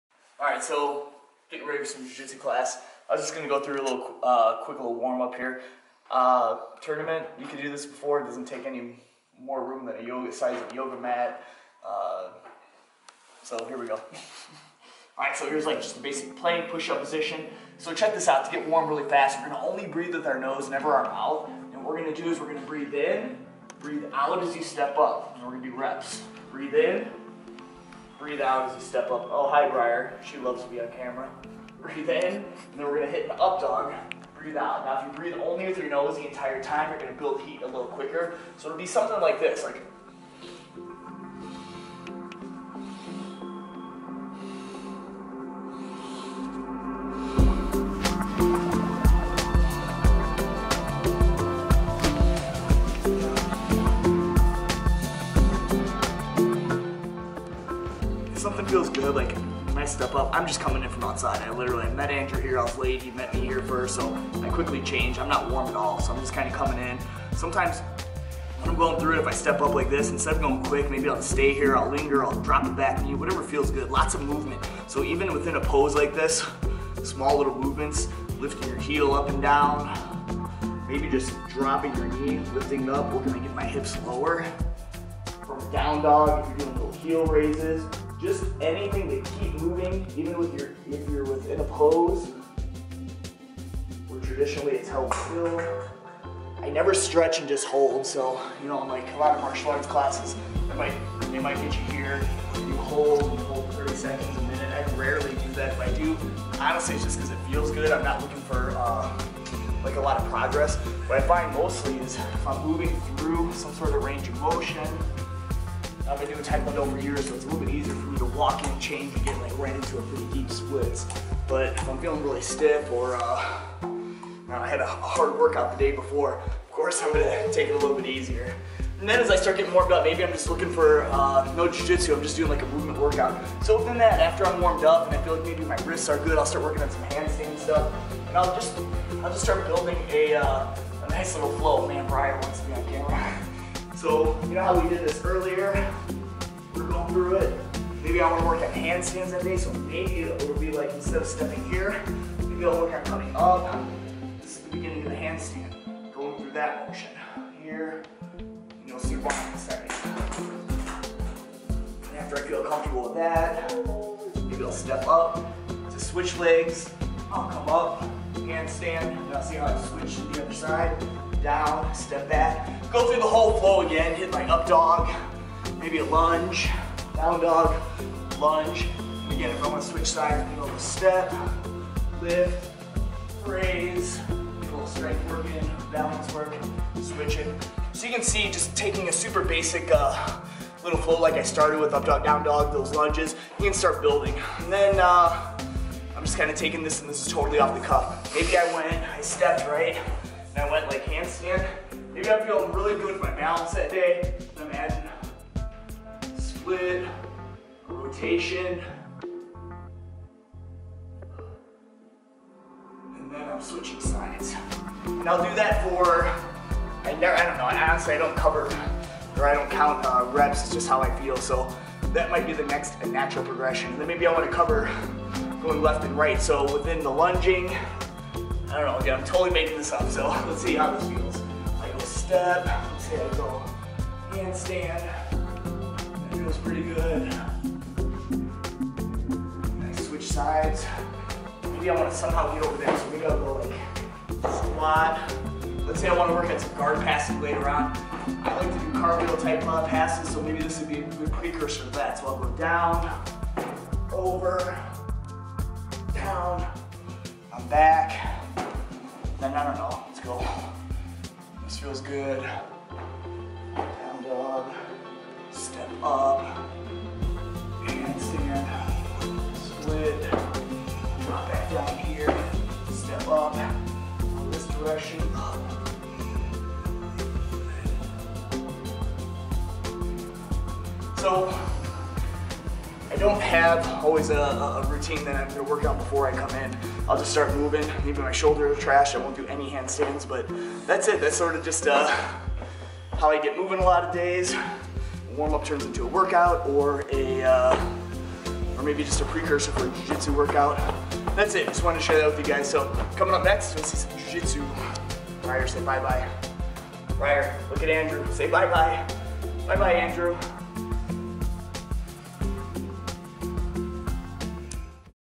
Movement Flow for Beginners to Advanced BJJ Athletes
Check out a little Vlog of how I put together a Movement Flow for either a warm up or a full workout.

How do you improve your BJJ?